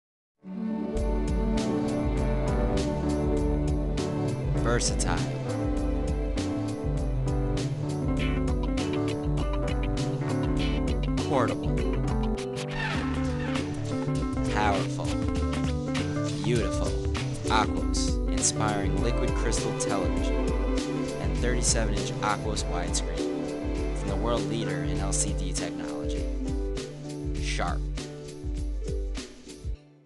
Sharp Electronics 1st LCD TV Commercial
The worlds first mass produced LCD TV